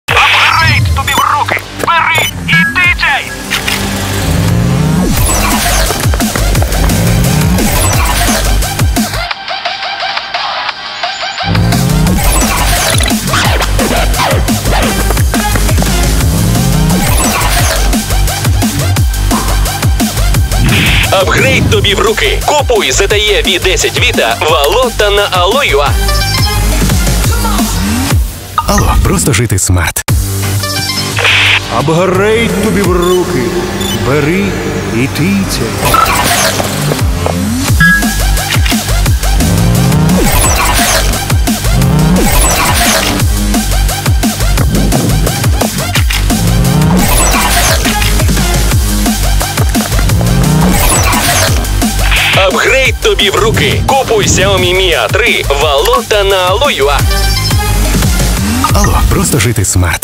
TABASCO создали промо-ролики для АЛЛО "Back to school"
“АЛЛО” в новых рекламных роликах от компании креатива TABASCO показала школьникам, как “апгрейднуться” перед новым учебным годом.

Волшебный трюк состоит в том, чтобы “тыцьнуть” на экран смартфона и решить все задачи одним кликом.

По сюжету, геймер попадает в реальную жизнь с прокачанным инструментом в руках, который позволяет моментально сделать level-up понравившегося “второстепенного персонажа”.

АЛЛО:
Артур Матковский — Chief Marketing Officer
Ольга Яшина — Head of Marketing Communications
Светлана Процив — Creative Head
Владислав Мисиюк —  Digital Head

TABASCO:
Александр Смирнов — Creative Director, Partner
Олег Завгородний — Associated Creative Director
Артем Гусев — Art Director
Анна Проничева — Copywriter
Татьяна Ильенко — Group Account Director
Екатерина Герасименко — Account Manager
Ольга Жегулина — Producer
Production — Clever Studios

Актеры:
Гуру — Игорь Вялов
Главный герой — Денис Систренский
Девушка — Алена Столярова
Музыкант — Андрей Асанов
Мальчик — Миша Черняков
Мама — Анели Азор
Папа — Роман Шерповал
Бабушка — Нина Галена